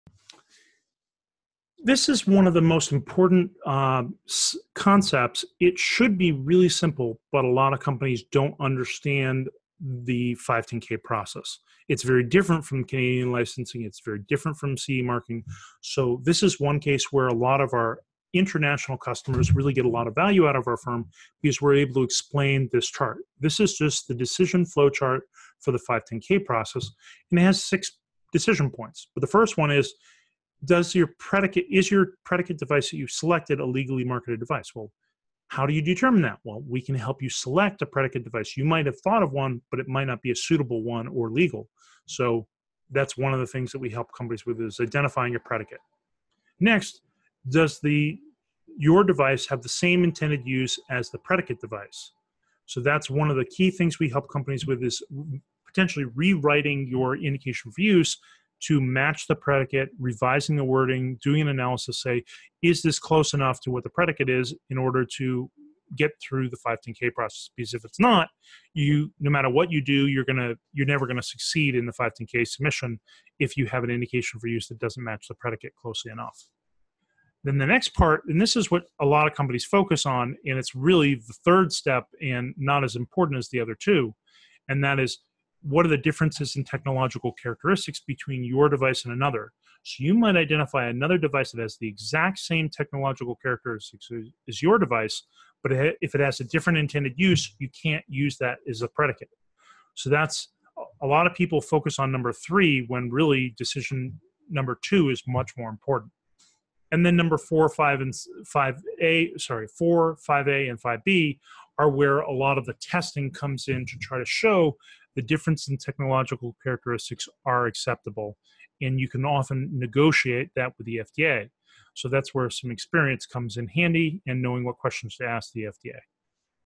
Substantial Equivalence in a Predicate Device
Substantial Equivalence is a unique concept to 510(k) Submissions to the FDA. This type of regulatory submission differs widely from CE Marking, and Canadian Licensing. Robert Packard explains some of the key factors and descision making considerations for determining if a predicate device is truly substantially equivalent for FDA Submissions.

We offer assistance with 510(k) and De Novo submissions to the FDA. Visit us at;